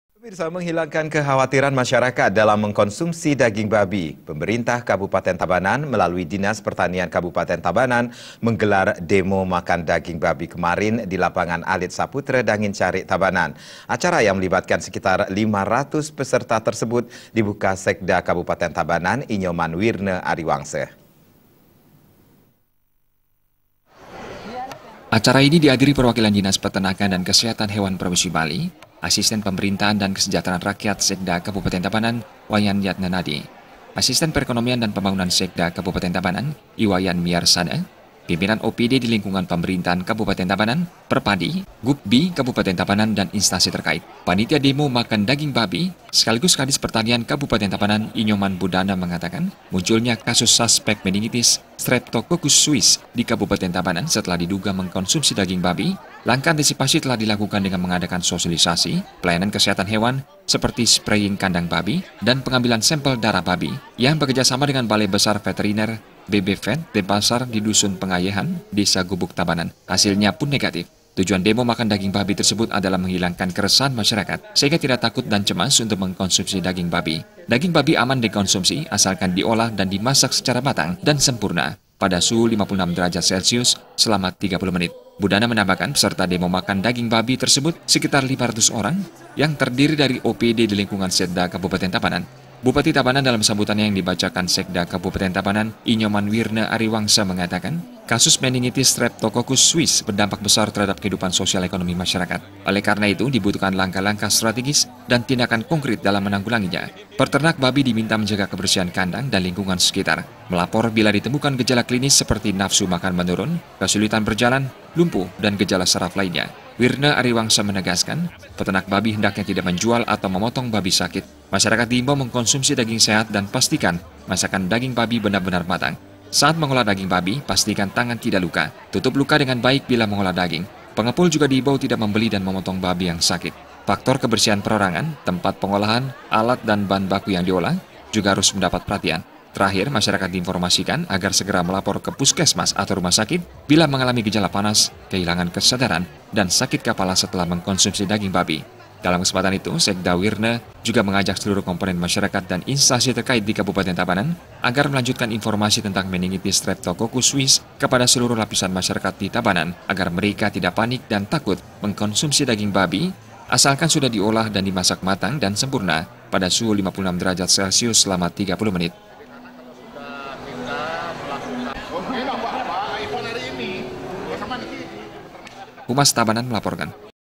SOSIALISASI DAGING BABI AMAN DIKONSUMSI
MENGHILANGKAN KEKHAWATIRAN MASYARAKAT DALAM MENGKONSUMSI DAGING BABI, PEMERINTAH KABUPATEN TABANAN MELALUI DINAS PERTANIAN KABUPATEN TABANAN MENGGELAR DEMO MAKAN DAGING BABI KEMARIN, DI LAPANGAN ALIT SAPUTRA DANGIN CARIK - TABANAN. ACARA YANG MELIBATKAN SEKITAR 500 PESERTA TERSEBUT DIBUKA SEKDA KABUPATEN TABANAN, I NYOMAN WIRNA ARIWANGSA.